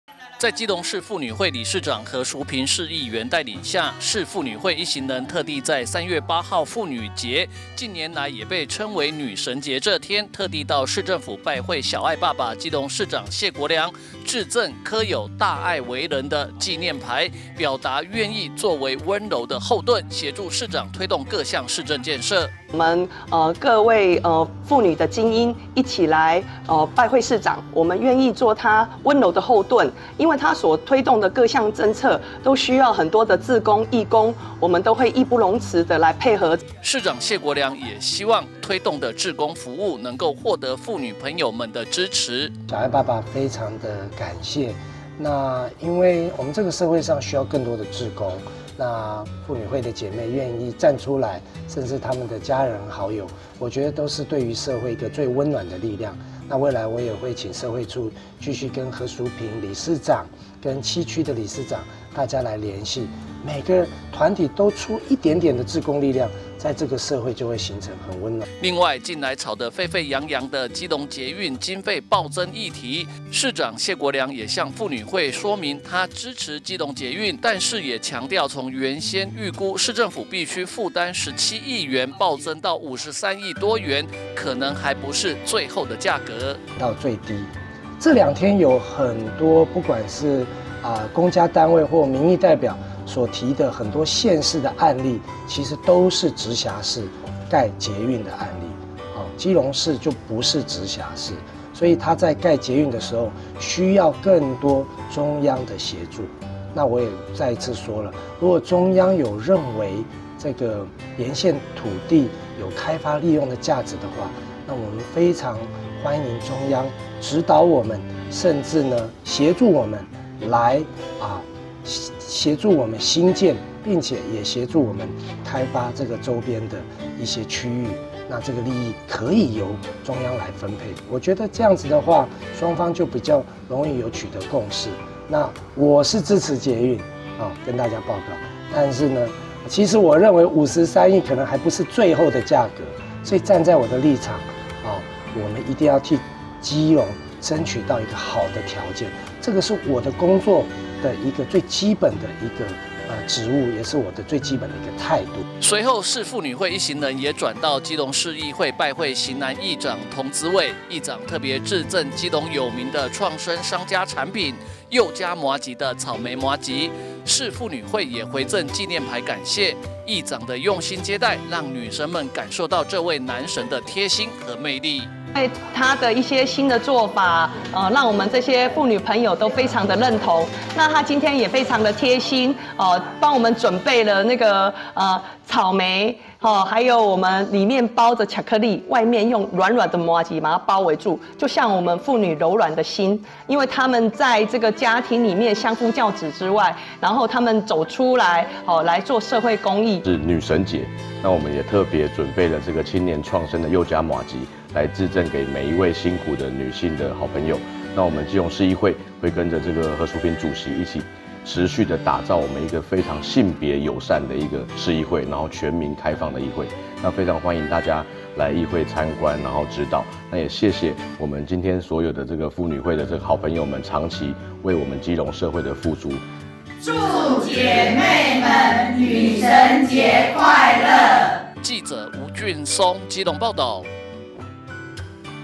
基隆|謝國樑|童子瑋|何淑萍|婦女節|女神節|基隆捷運|志工|婦女會|女神節 市婦女會拜訪市長 願做市政溫柔後盾
女神節 市婦女會拜訪市長 願做市政溫柔後盾
 女神節 市婦女會拜訪議長 感受男神貼心接待

(中嘉吉隆新聞記者吳俊松/基隆報導)3月8號婦女節，也有人稱為”女神節”，基隆市婦女會特地到市政府拜會小愛爸爸基隆市長謝國樑，表達願意做為”溫柔的後盾”協助市長推動市政建設，隨後市婦女會一行人也轉到基隆市議會拜會型男議長童子瑋，議長的用心接待讓女神們感受到這位男神的貼心和魅力！

在基隆市婦女會理事長何淑萍議員帶領下，市婦女會一行人特地在3月8號婦女節，近年來也被稱為”女神節”這天，特地到市政府拜會小愛爸爸基隆市長謝國樑，致贈刻有”大愛為仁”的紀念牌，表達願意做為”溫柔的後盾”協助市長推動各項市政建設！

市長謝國樑也希望推動的志工服務能夠獲得婦女朋友們的支持！

另外，近來炒得沸沸揚揚的基隆捷運經費暴增議題，市長謝國樑也向婦女會說明他支持基隆捷運，但是也強調，從原先預估市政府必須負擔17億元暴增到53億多元可能還不是最後的價格！

隨後市婦女會一行人也轉到基隆市議會拜會型男議長童子瑋，議長特別致贈基隆有名的創生商家產品”祐家麻吉”的草莓麻吉，市婦女會也回贈紀念牌感謝，議長的用心接待讓女神們感受到這位男神的貼心和魅力！